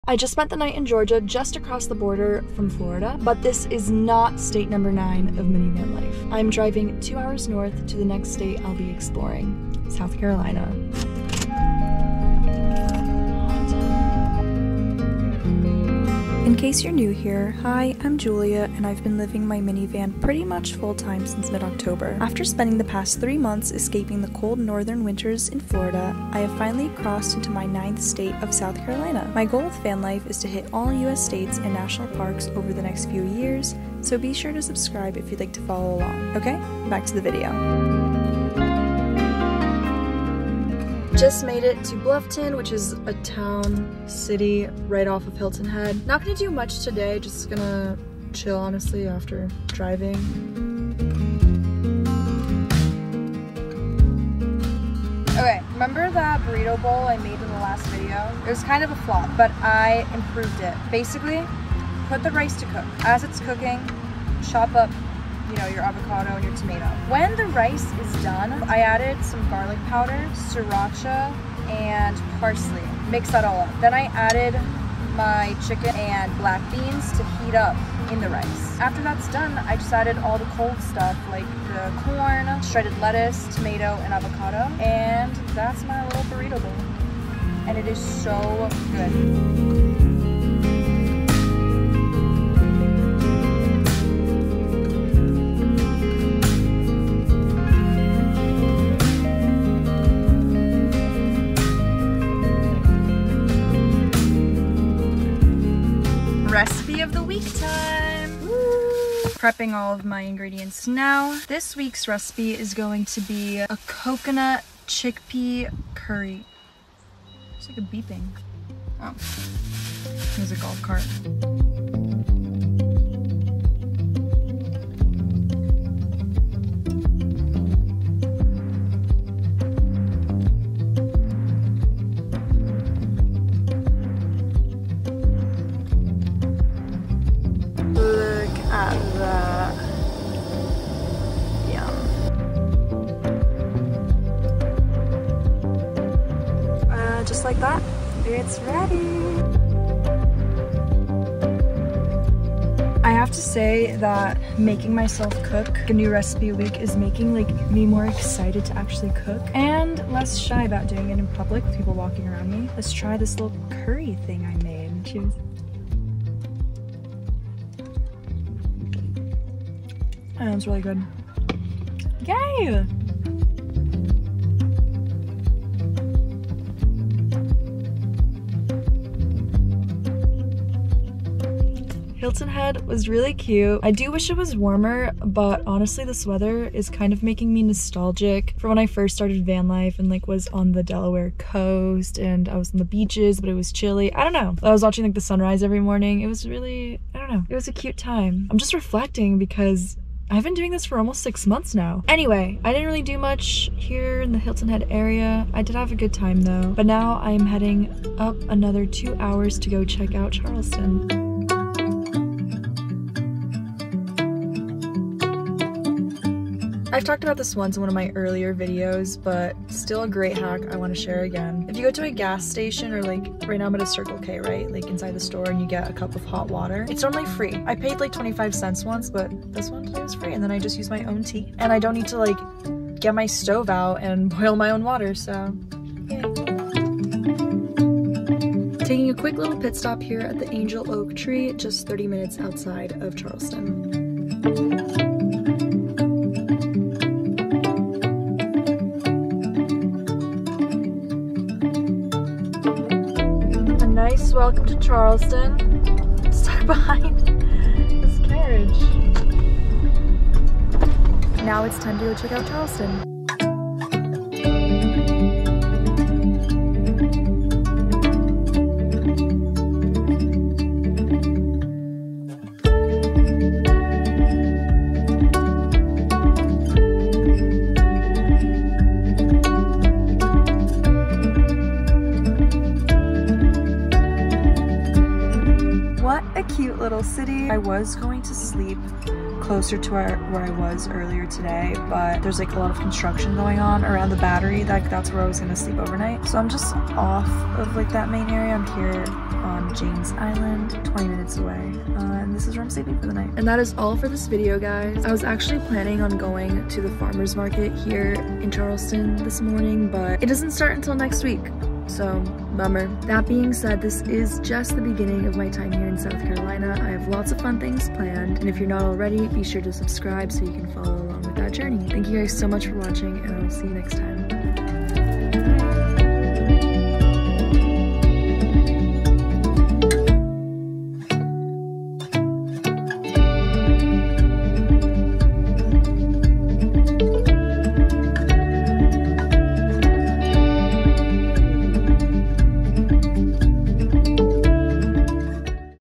state #9 of minivan life!
A short & chill video of my first two stops in SOUTH CAROLINA: Hilton Head & Charleston.

Chapters:
00:00 driving to hilton head
00:41 hilton head
03:37 driving to charleston
04:21 charleston